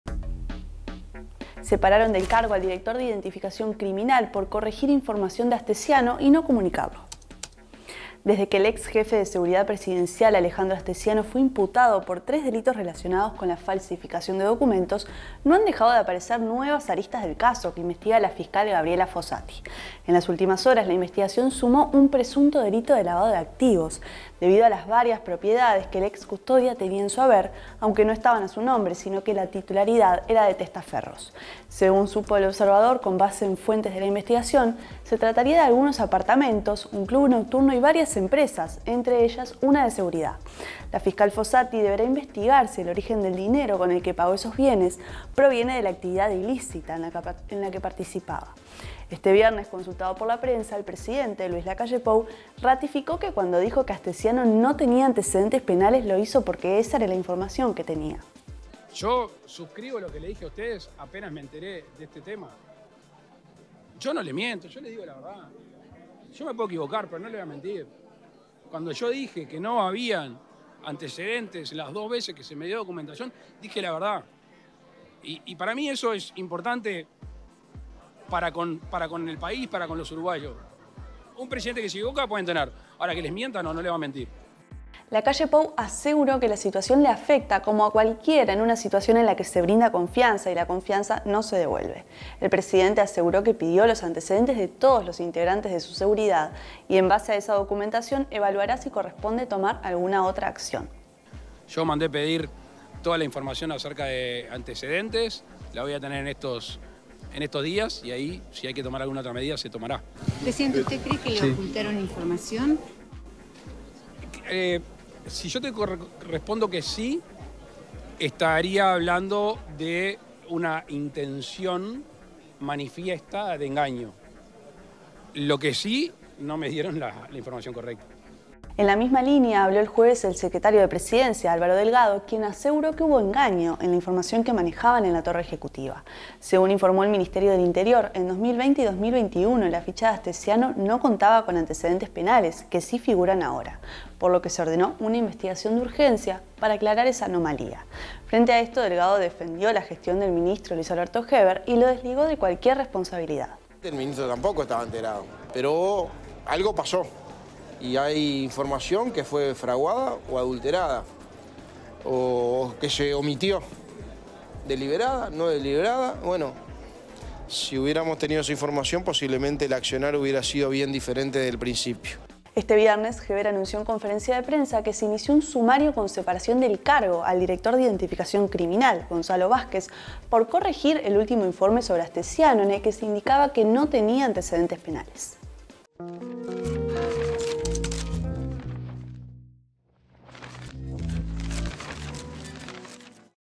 Separaron del cargo al director de Identificación Criminal por “corregir” información de Astesiano
la diaria vivo: Separaron del cargo al director de Identificación Criminal por “corregir” información de Astesiano y no comunicarlo